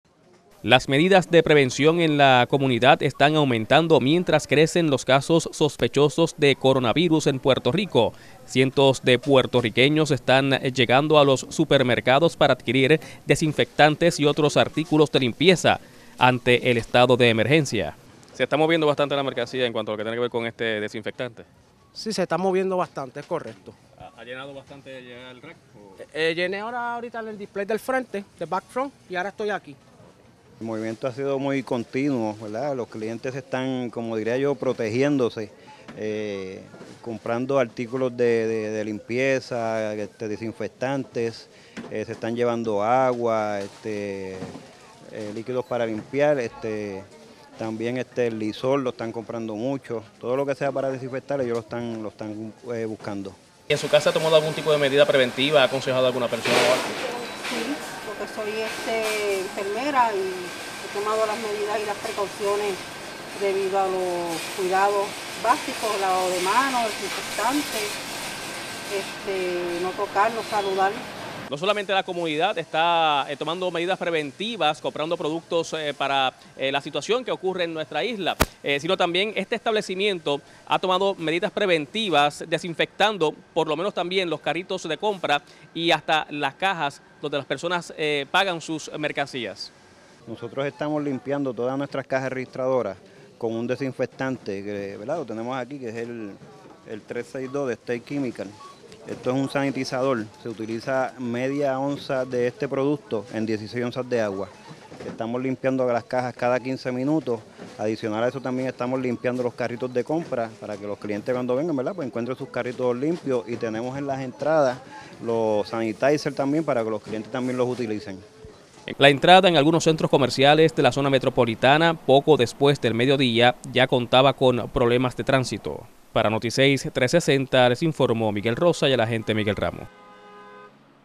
Boricuas abarrotan los comercios en busca de productos de higiene y limpie
Supermercados y centros comerciales en la isla están siendo visitados por la ciudadanía en busca de productos desinfectantes y de limpieza ante la emergencia que se vive en Puerto Rico con el aumento de casos sospechosos del virus COVID - 19. El agente Miguel Ramos nos trae la información.